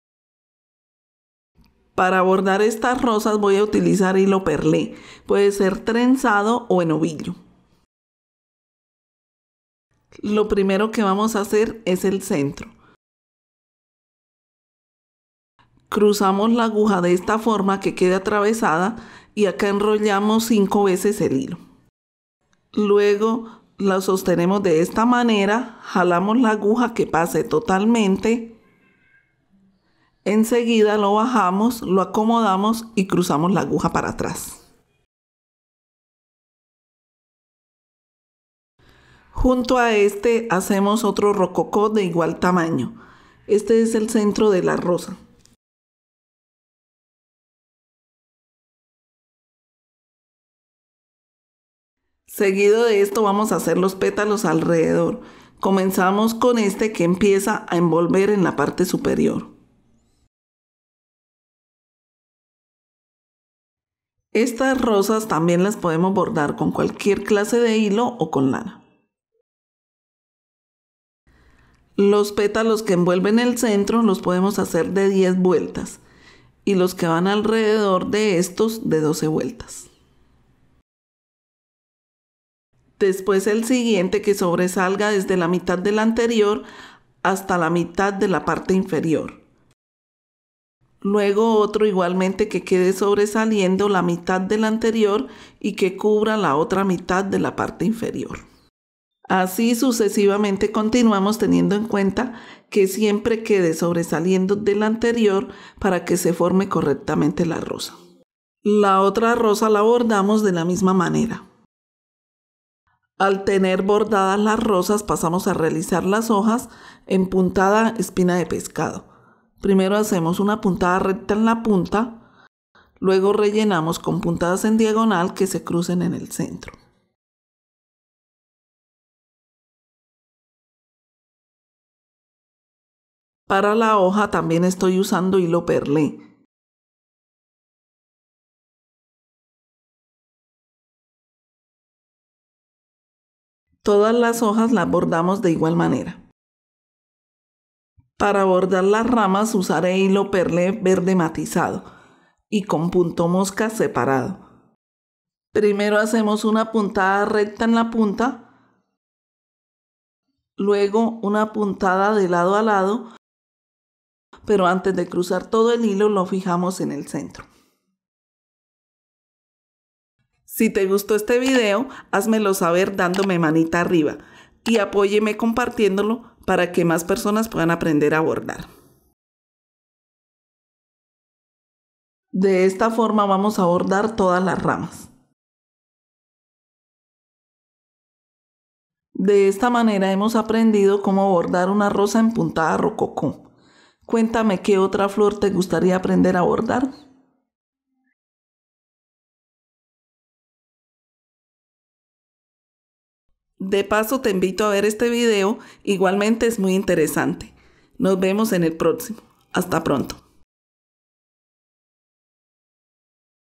HERMOSO BORDADO DE ROSAS EN ROCOCÓ
¡Hola a todos! Hoy te muestro cómo bordar Rosas en Rococó. Es una técnica que podrás realizar fácilmente. Puedes aplicarla para distintos proyectos como un mantel, un centro de mesa, una servilleta o para un cuadro decorativo. Las aplicaciones son innumerables. Espero te guste.

Tutorial de bordado a mano por Artesd'Olga